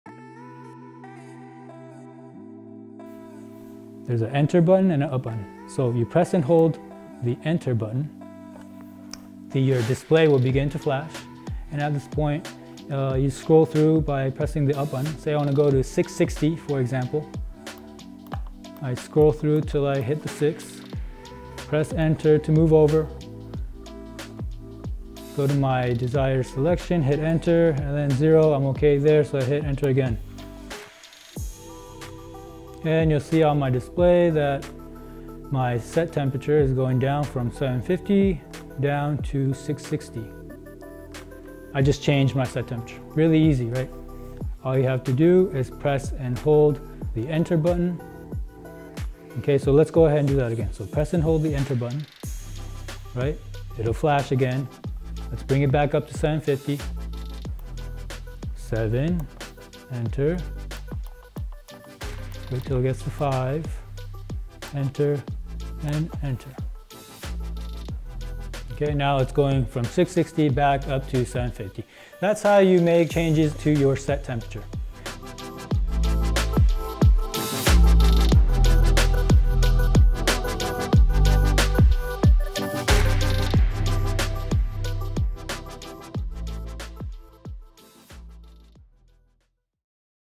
How to Set the Temperature on the FX-888D Soldering Station
A quick tip video on how to set the temperature on the FX-888D Soldering Station. Hold the enter button until the first digit blinks, then use the arrow button to select your desired temperature.